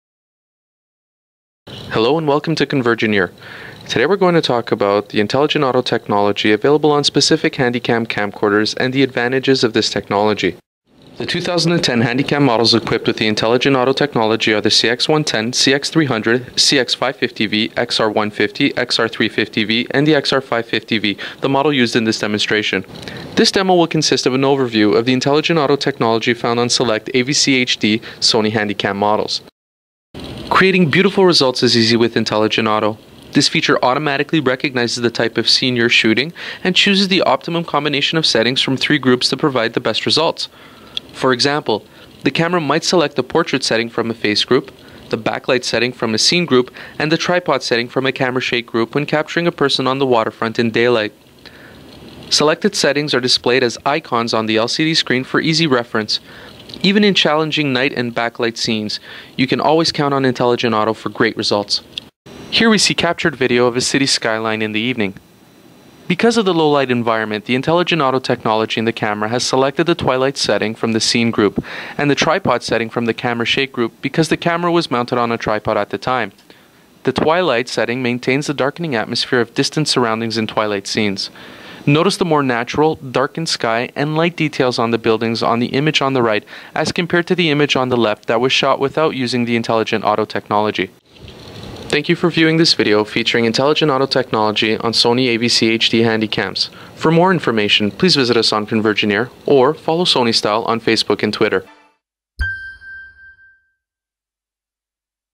Sony Handycam Intelligent Auto Overview
Check out this video to learn about the Sony Handycam Intelligent Auto or iAuto feature.

Creating beautiful results is easy with Intelligent Auto. This feature automatically recognizes the type of scene you are shooting and chooses the optimum combination of settings from three groups to provide the best results. For example, the camera might select the Baby setting from the Face group, the Low Light setting from the Scene group and the Tripod setting from the Camera-shake group when capturing a baby indoors. Selected settings are displayed as icons on the LCD screen for easy reference. Even in challenging night and backlit scenes, you can always count on Intelligent Auto for great result